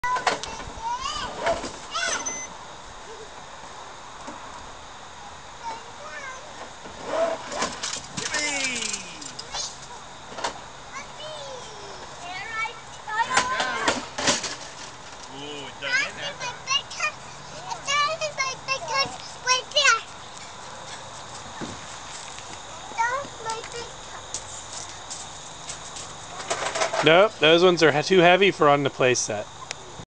Monte and Warren on their Playhouse
Monte and Warren play with Pappy on the playhouse, pushing cars down the slide.